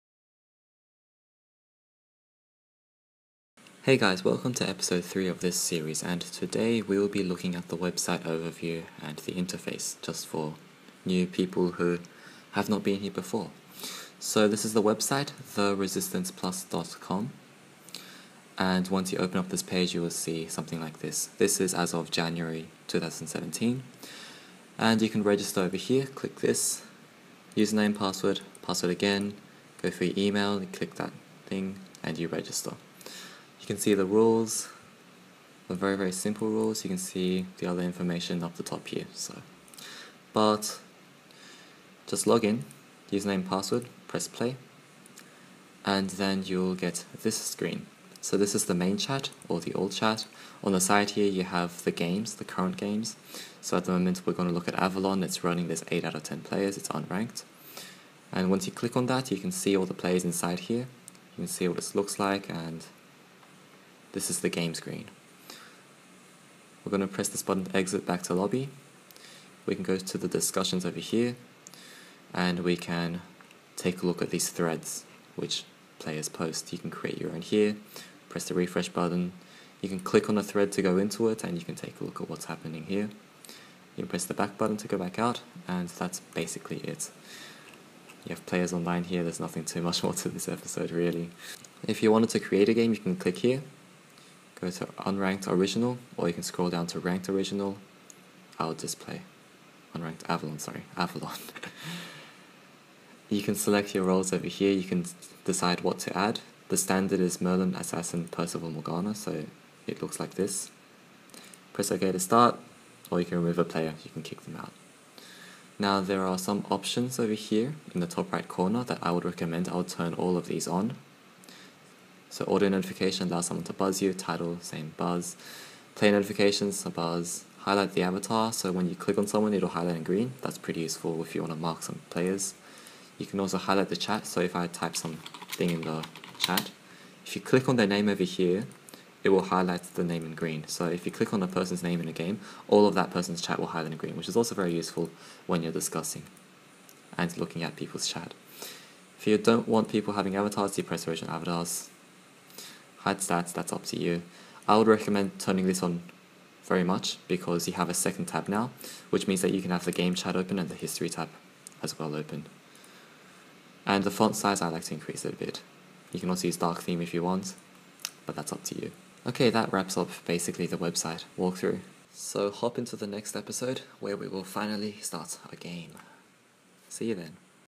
Ep 3 - Website overview
The Resistance!

This episode will quickly cover the website interface to teach you how to navigate the website.